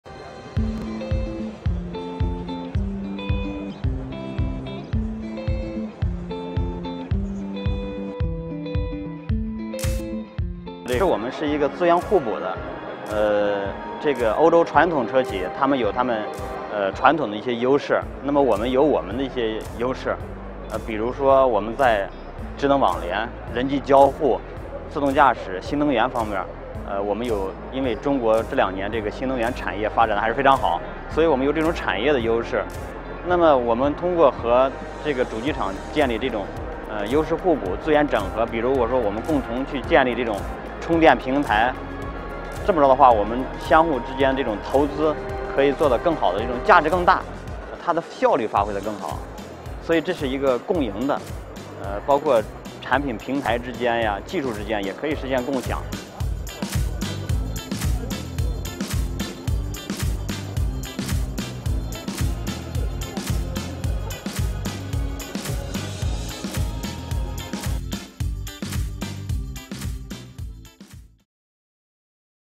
GLOBALink | Chinese automakers ready for win-win cooperation with European counterparts: Chinese CEO
Chinese and European auto firms should take advantage of their complementary roles in the industry to achieve win-win outcomes, chief executive of the Chinese carmaker Great Wall Motor (GWM) Europe has said.